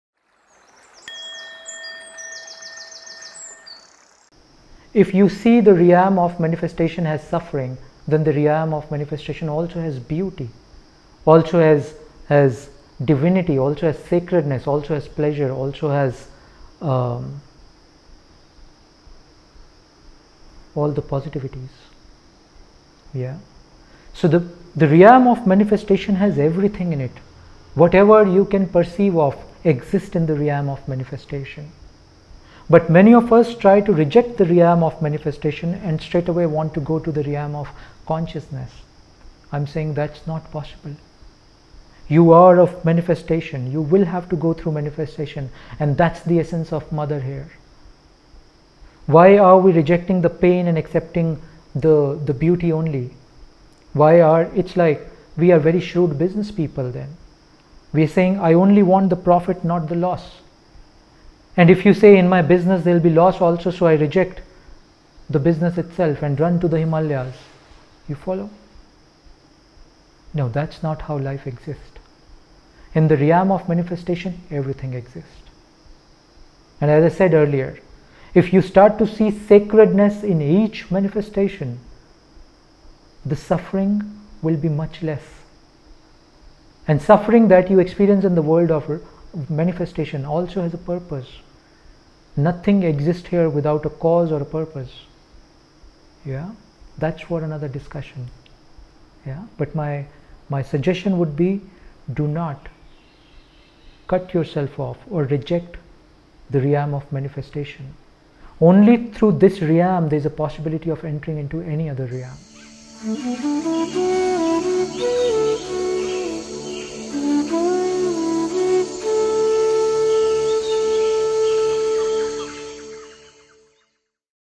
Why there is so much suffering in the world? | Talks with Sri Anish
A friend asked, is there only suffering in the manifested world? why is it so? Sri Anish shares a beautiful perspective in this 2 mins. video.